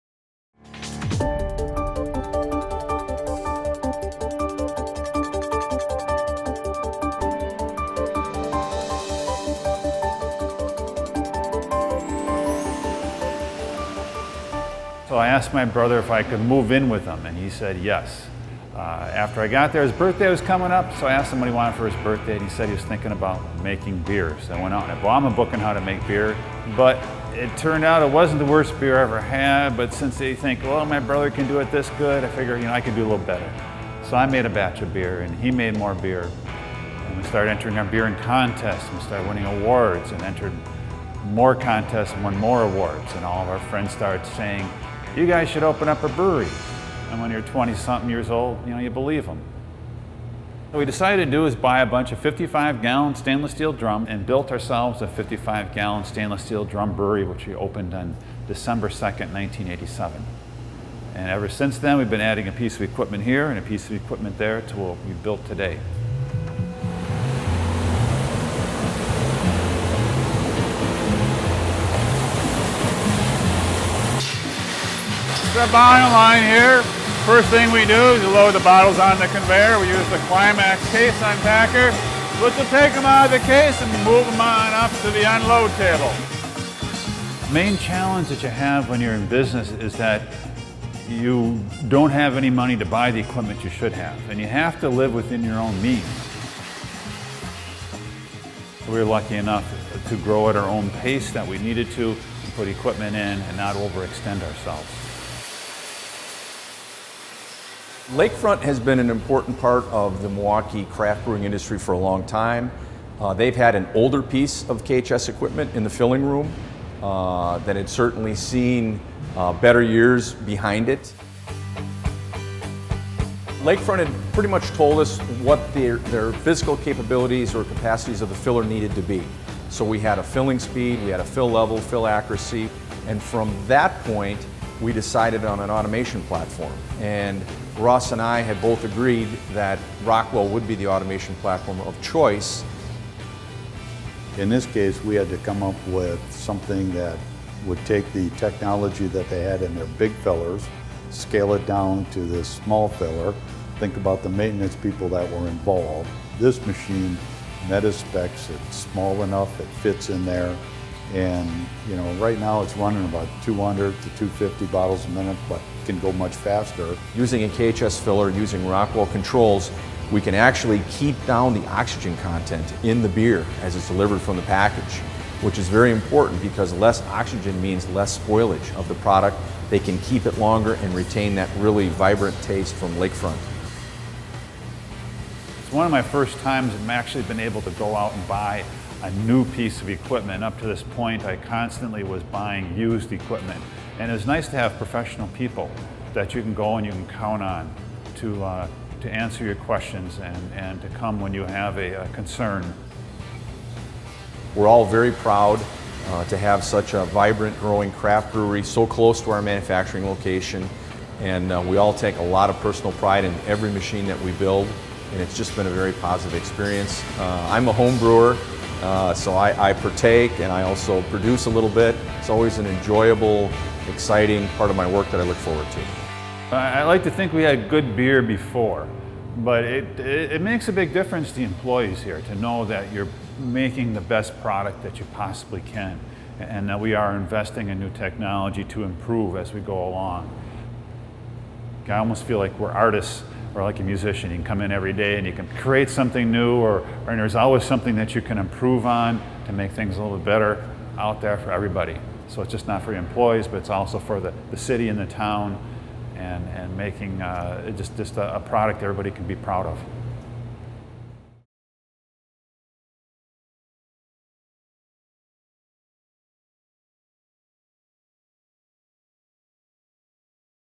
Craft Brewer Increases First-Pass Yield and Throughput with New Filling Equipment
Lakefront Brewery in Milwaukee, which started brewing craft beers in 1986, upgraded its existing filling equipment with new technology from KHS USA Inc. and Rockewell Automation. The improved system increased production speed from 120 to 180 bottles per minute and improved quality with 80 to 90 percent error reduction in measuring parts per million of oxygen.